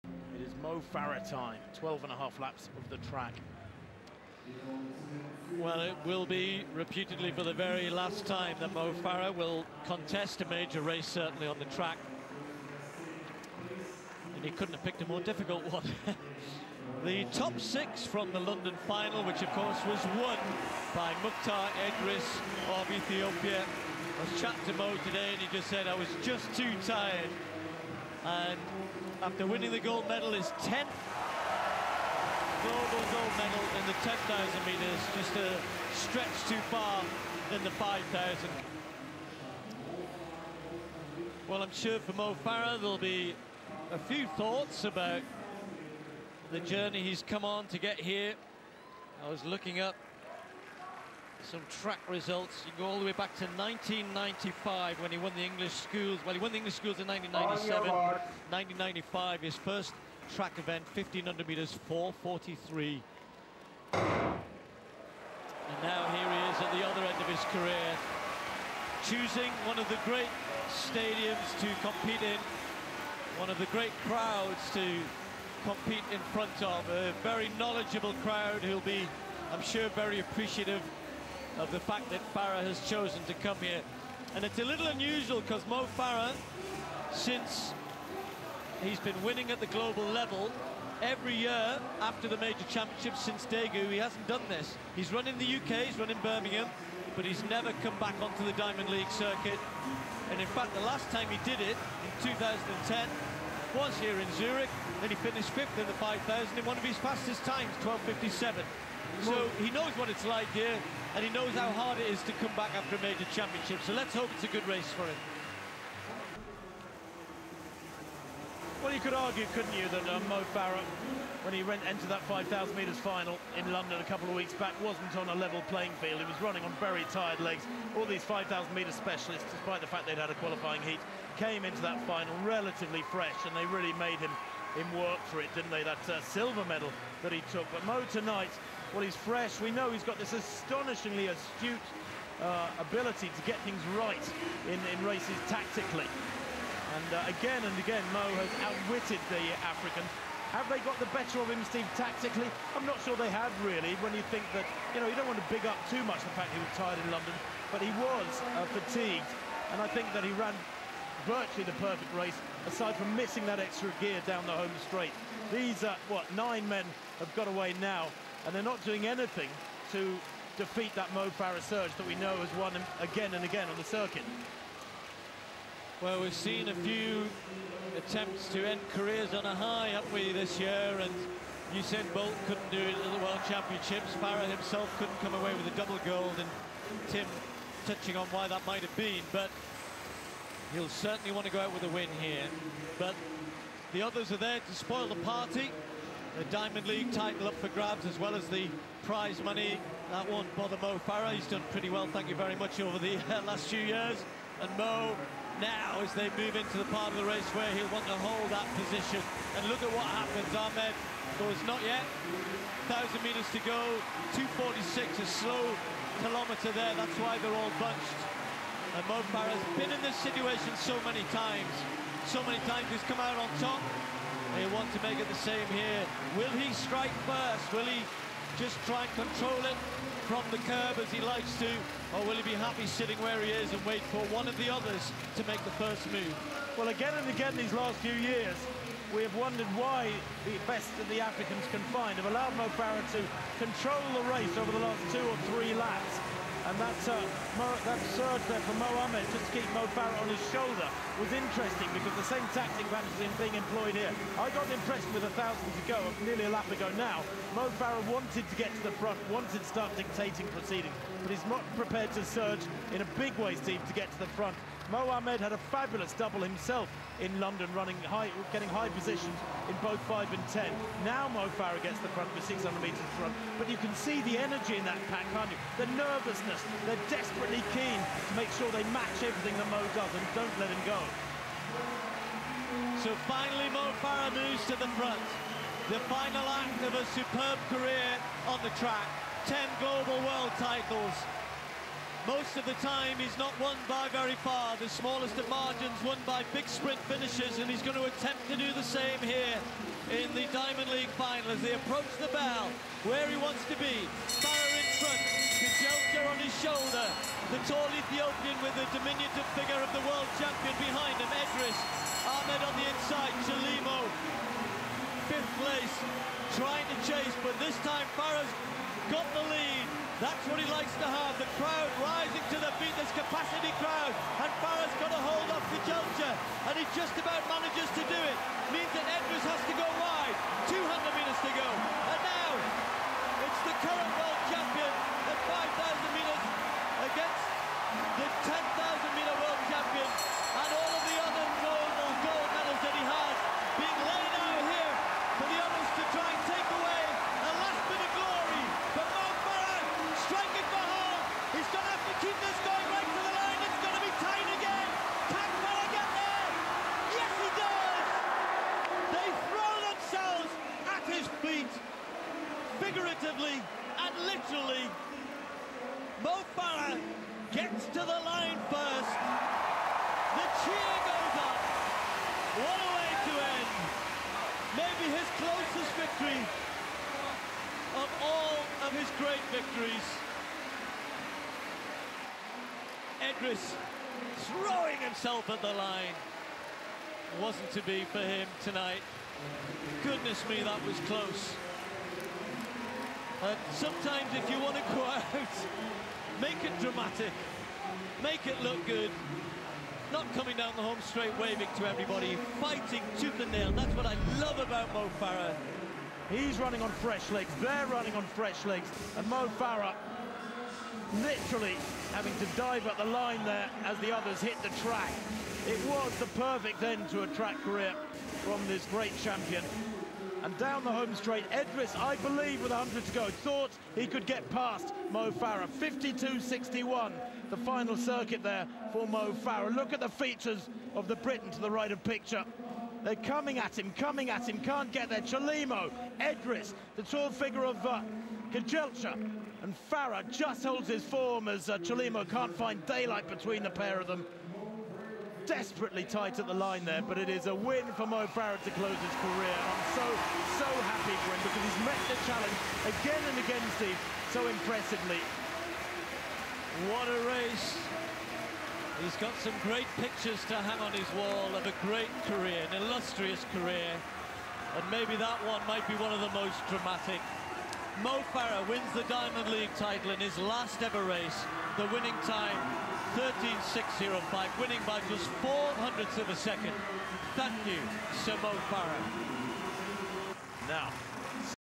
Mo Farah 5000m, Zurich 2017
Mo Farah's last track race.